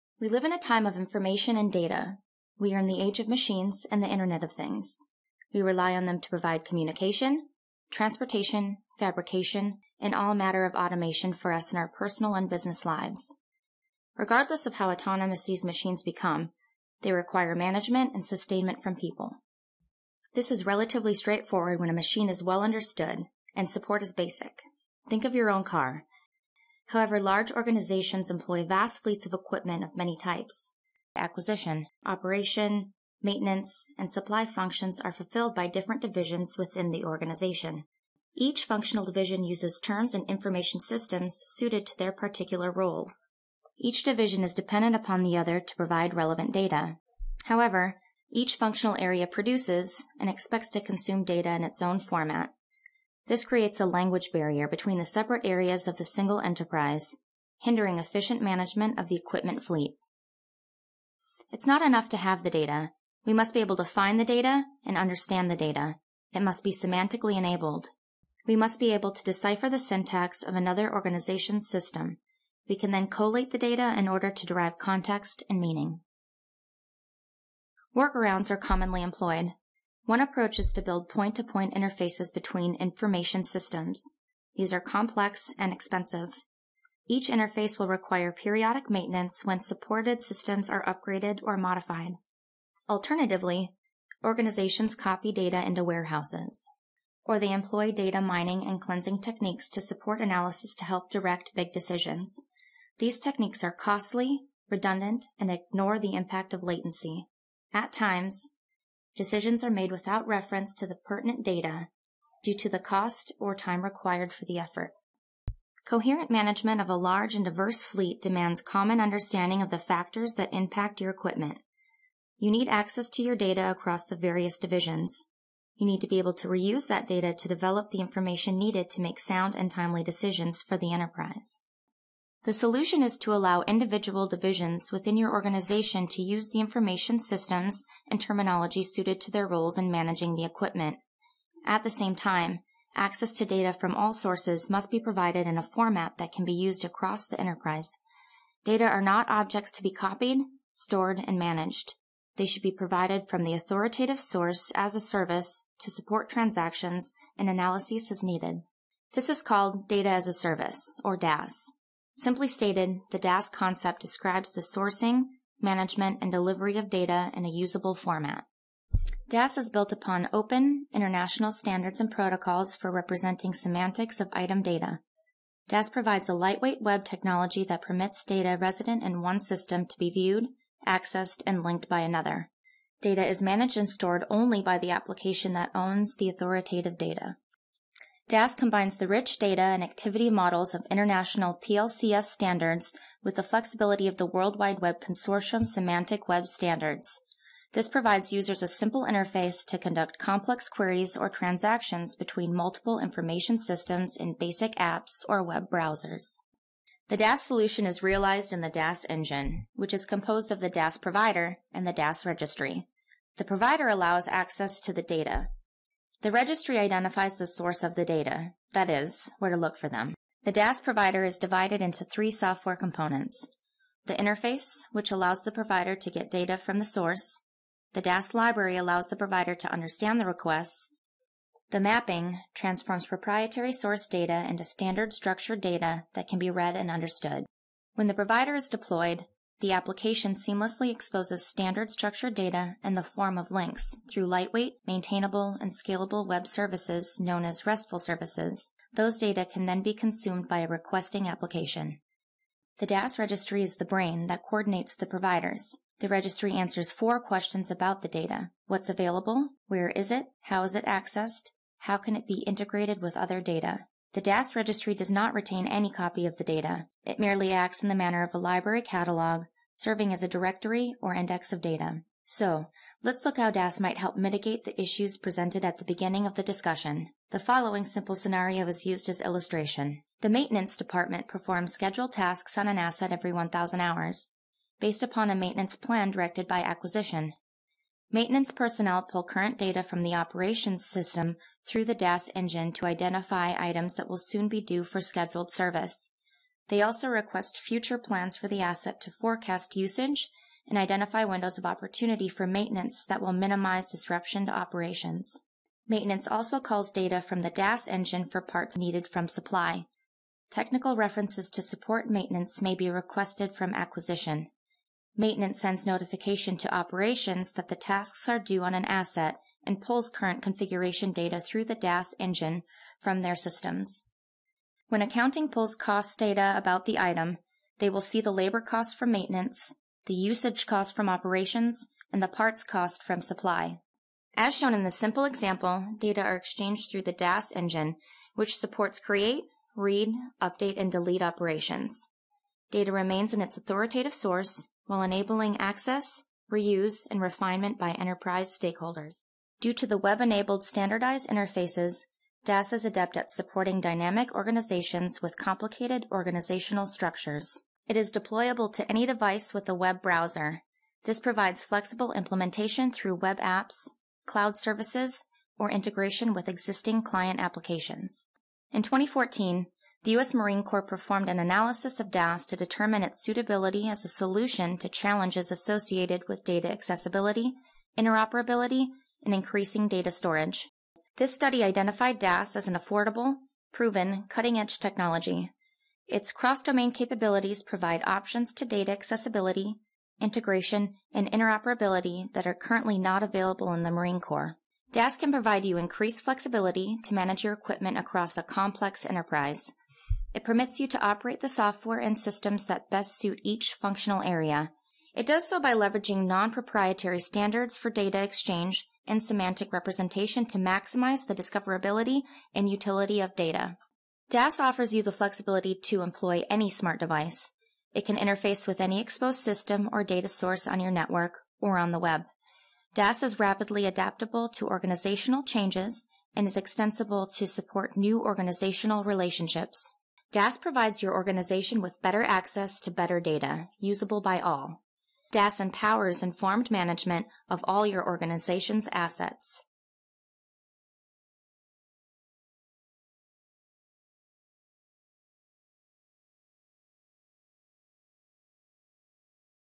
Data as a Service (DaaS)
Nexus Life Cycle Management and Engisis bring you a tutorial on Data as a Service and how it can resolve common data and system interoperability issues when managing equipment across organizations.